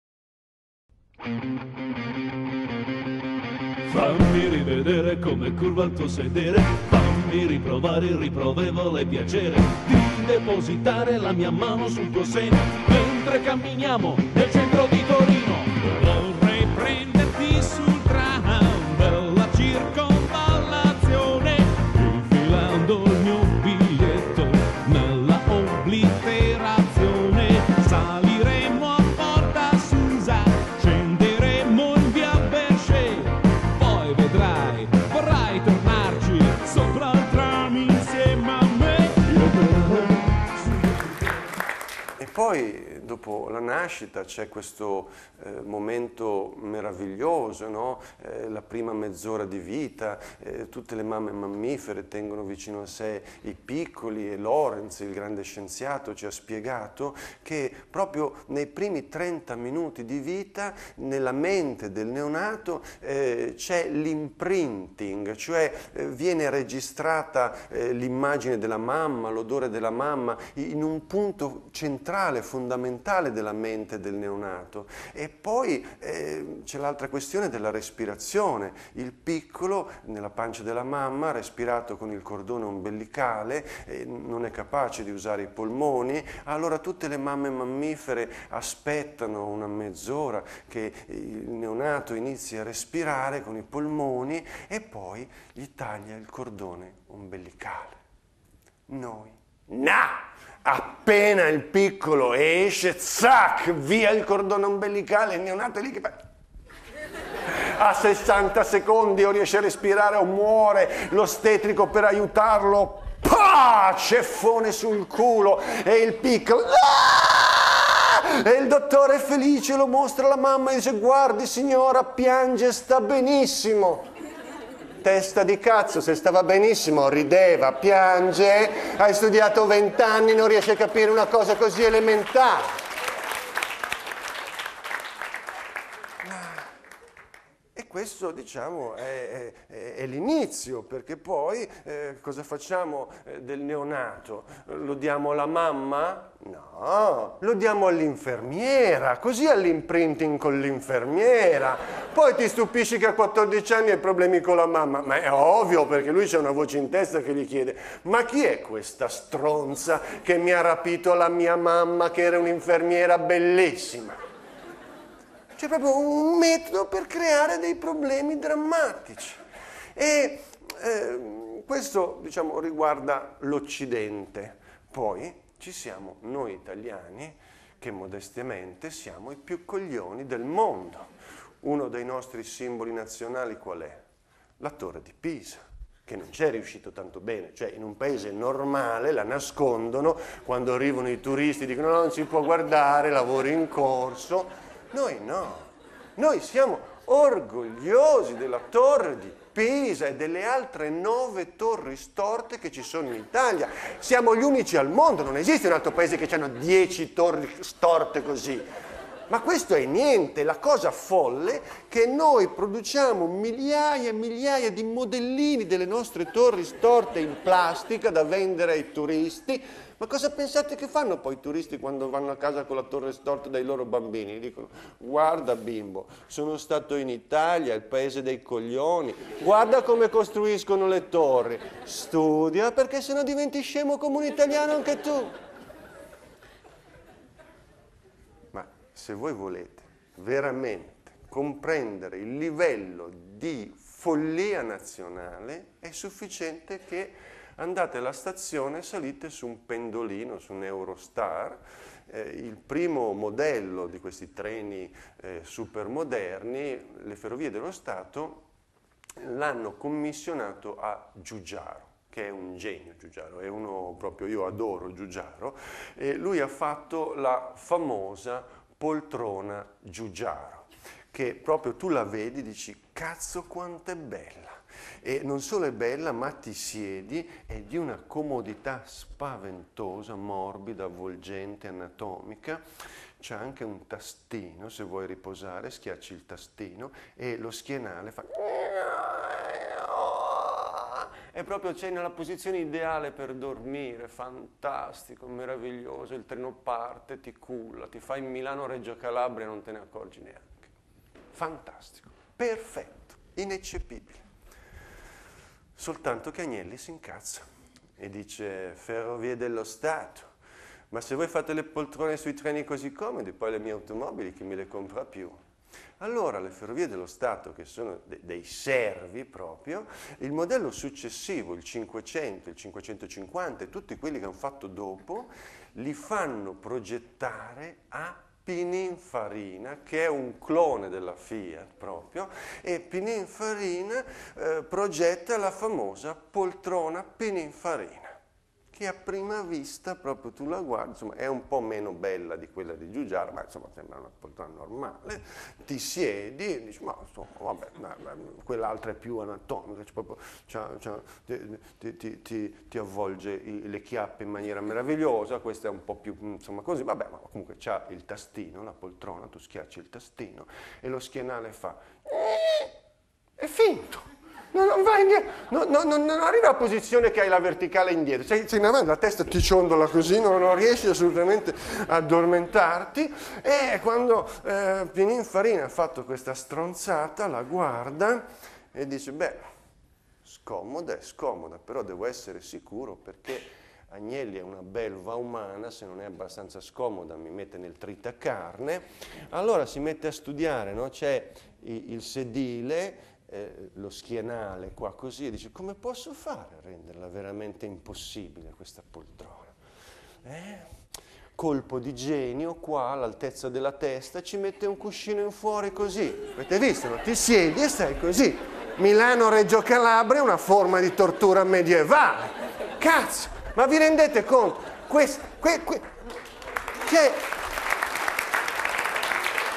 If you have sex with elephants never lie underneath - pt.2
If you have sex with elephants never lie underneath
Recital of pieces from:
- Zen and the art of making love
- I love you but your right arm disgusts me. Cut it off.
- Even soles (fish) fake orgarms. Imagine all the rest.
Rome "Teatro dei Satiri" - March 17th and 18th 2006
Production: Eleonora Albanese